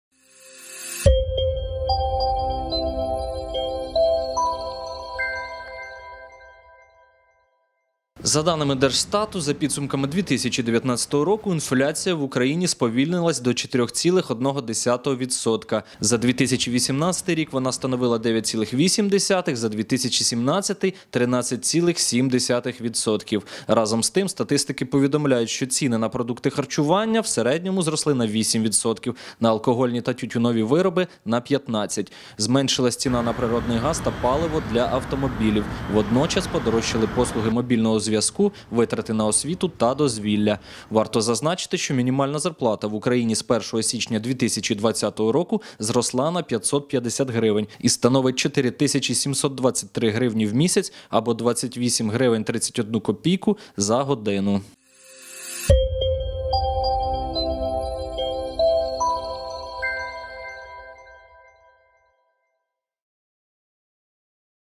Інфляція сповільнюється.
Державна служба статистики повідомляє — інфляція в Україні знижує темпи. Водночас дорожчають харчові продукти.